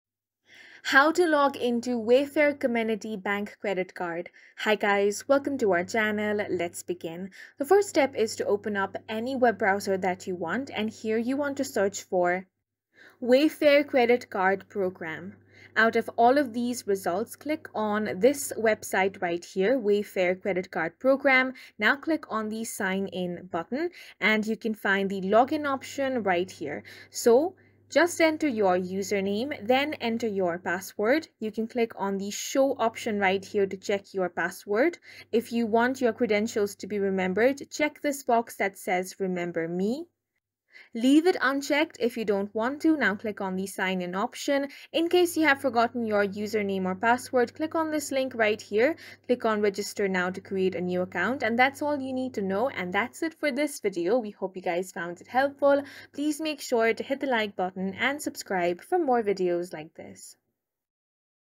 How to Login WayFair Comenity Bank Credit Card | Wayfair Credit Card Login for MasterCard Payment
Are you looking for guidance on how to log in to your WayFair Comenity Bank Credit Card account? You're in the right place! In this tutorial, we'll provide step-by-step instructions to help you access your credit card account seamlessly.

Introduction:
Learn the importance of logging in to your WayFair Comenity Bank Credit Card account and the benefits it offers for managing your finances.

Step-by-Step Tutorial:
🕒 Timestamps:

[00:00] Introduction and Account Access Overview
[00:10] How to Login to WayFair Comenity Bank Credit Card

Our straightforward instructions will guide you through the login process, ensuring you can easily access and manage your credit card account.